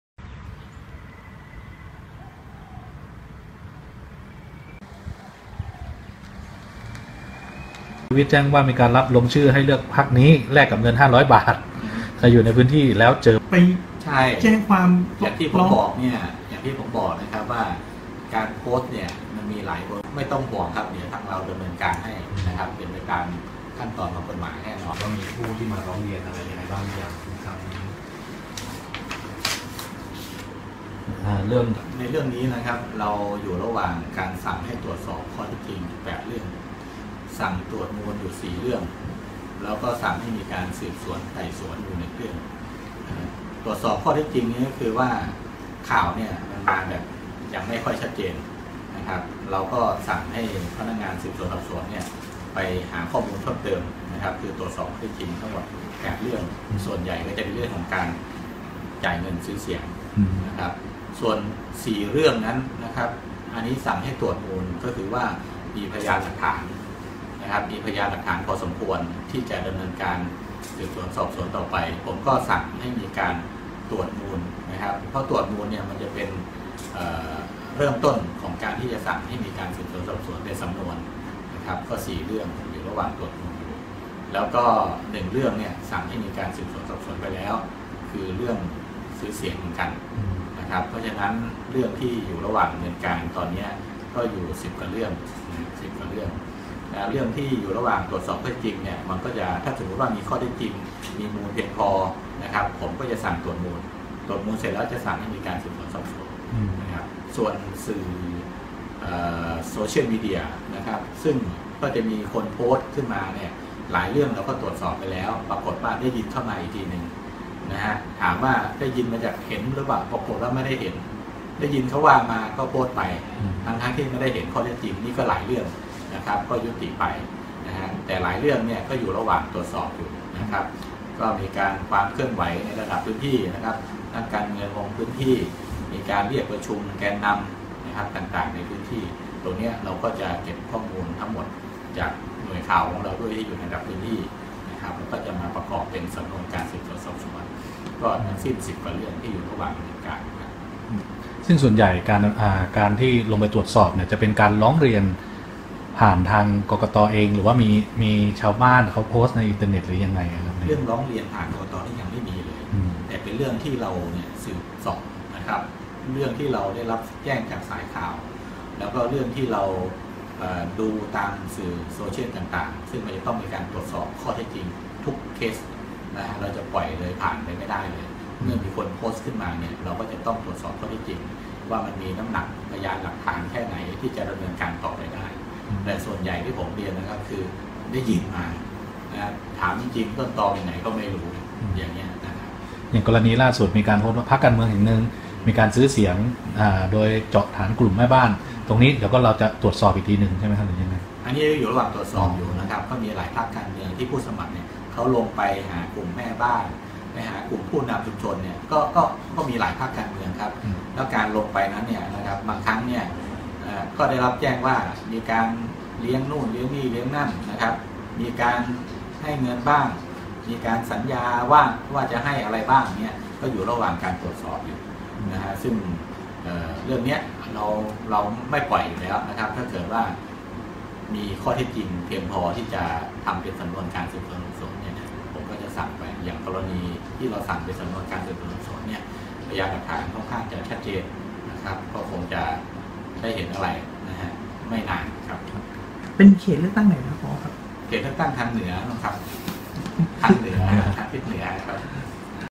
ผอ.กกต.เชียงใหม่แย้มเจอหลักฐานชัดผิดซื้อเสียงเขตเลือกตั้งโซนเหนือจ่อลงดาบ-พร้อมเร่งสอบเบาะแสโซเชียล
12/03/2019 ผอ.กกต.เชียงใหม่แย้มเจอหลักฐานชัดผิดซื้อเสียงเขตเลือกตั้งโซนเหนือจ่อลงดาบ-พร้อมเร่งสอบเบาะแสโซเชียล

ศูนย์ข่าวเชียงใหม่-ผอ.กกต.เชียงใหม่ สั่งตรวจสอบแล้วกรณีมีการโพสต์โซเชียลกล่าวหาผู้สมัคร ส.ส.ซื้อเสียง พร้อมเผยพบหลักฐานชัดจ่อลงดาบ1เรื่องซื้อเสียงเขตเลือกตั้งโซนเหนือ